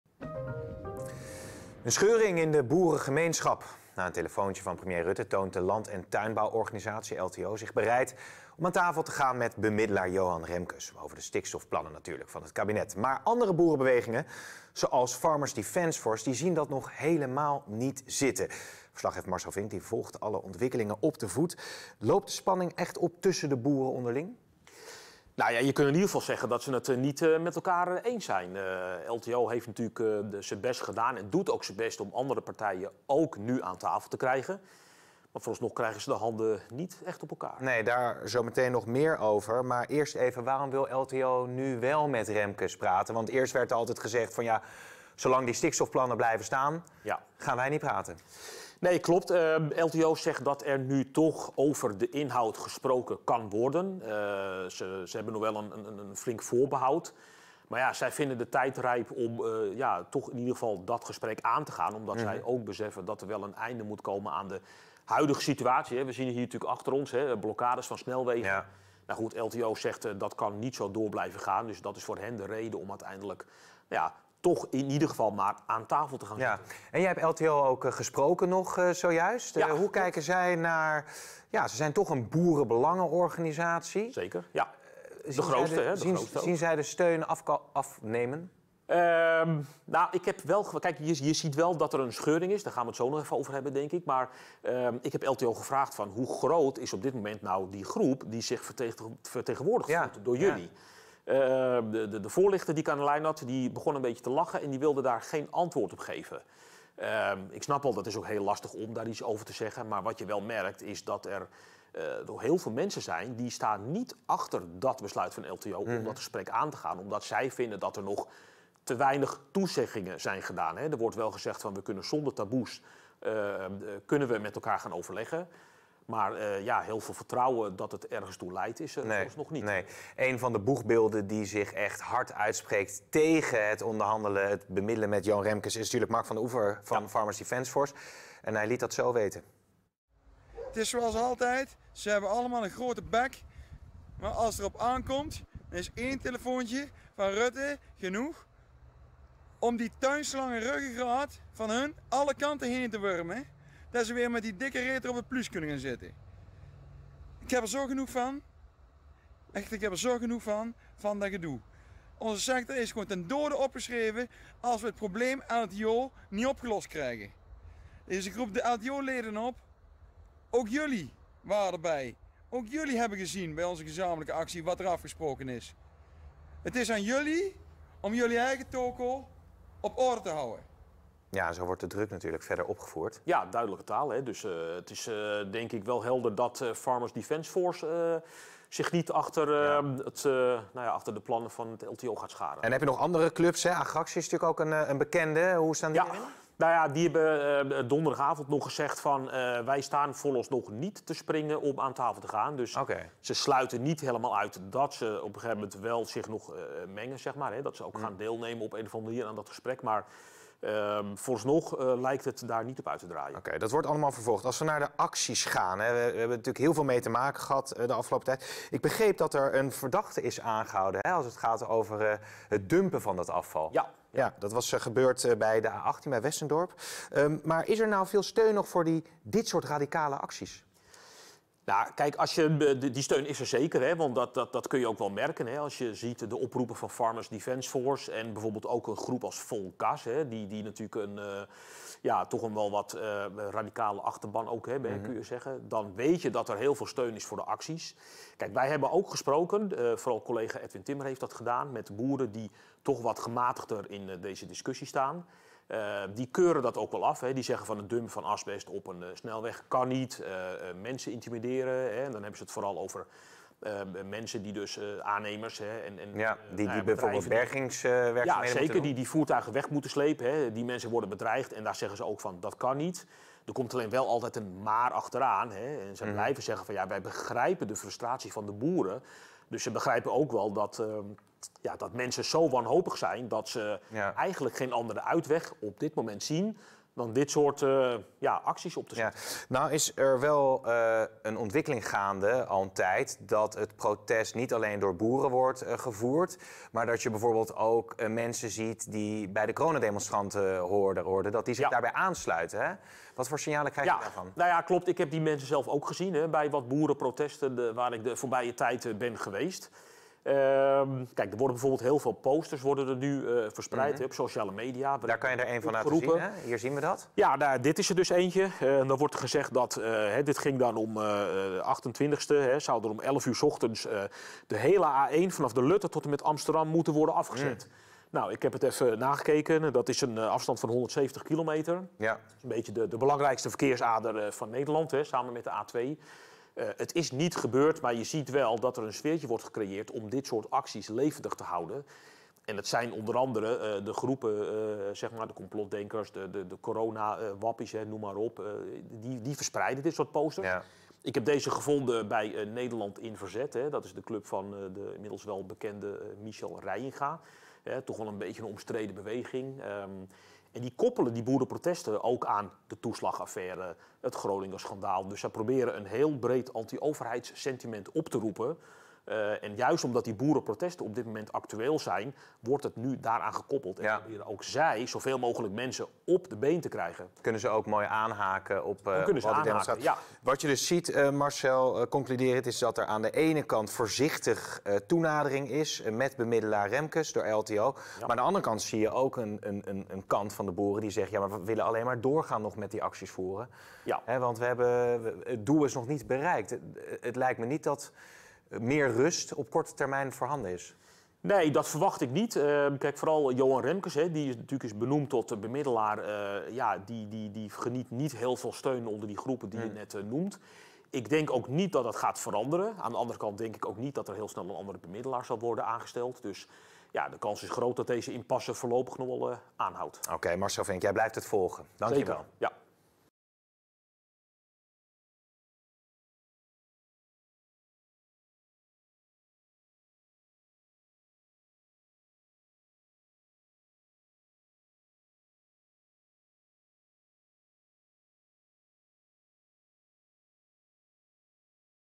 ‘Omstreden beweging mengt zich in boerenprotesten’
Er ontstaat een scheuring in de boerengemeenschap nadat LTO zich bereid heeft getoond om met bemiddelaar Johan Remkes om te tafel te gaan. Op de achtergrond speelt echter nog meer rond de boerenprotesten, weet verslaggever Marcel Vink.